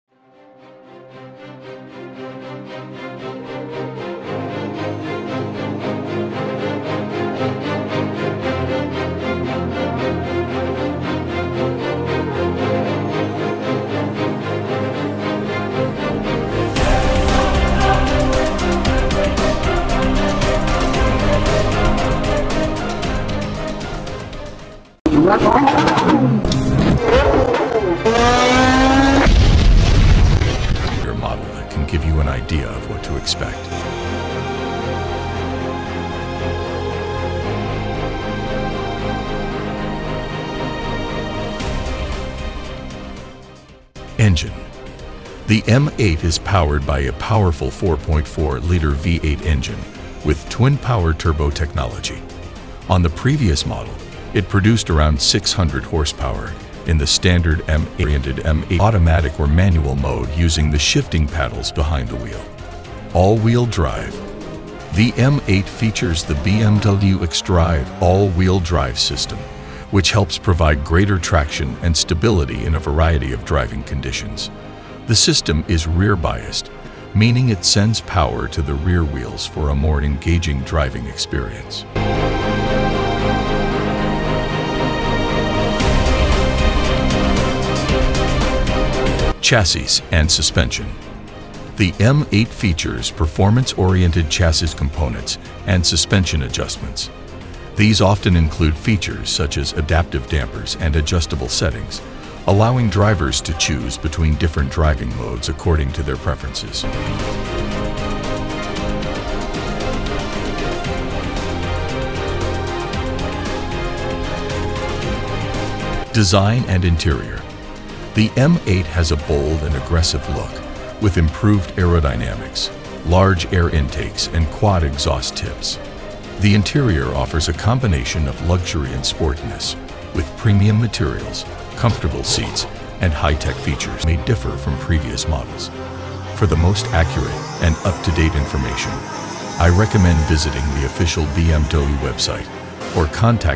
BMW M8 2023: Unleashing Power and Elegance on the Road"
Overview :
The mind-bending acceleration delivered by the BMW M8’s 617-hp twin-turbo V-8 is strong enough to get the car to 60 mph in just 2.5 seconds. At 10.7 seconds the M8 reaches 129 mph as it covers a quarter-mile of road. That’s quicker than a 710-hp Ferrari 488 Pista from something with a rear seat weighing more than two tons. Chalk it up to all-wheel drive traction and an overachieving engine. The M8 is crazy-fast, but its smooth delivery of felony speeds and its bolted-to-the-road stability make driving fast feel almost responsible. Available as both a coupe and convertible (the four-door M8 Gran Coupe is reviewed separately), the M8 cockpit is well-appointed and uses a large infotainment screen to handle navigation or audio selection. However, its rear seat isn’t a practical space. As one of the most affordable cars that will get you into 10-second quarter miles every time you try, the M8 is a big coupe with even bigger performance numbers.
On this channel I will describe a little information about the latest vehicles / cars.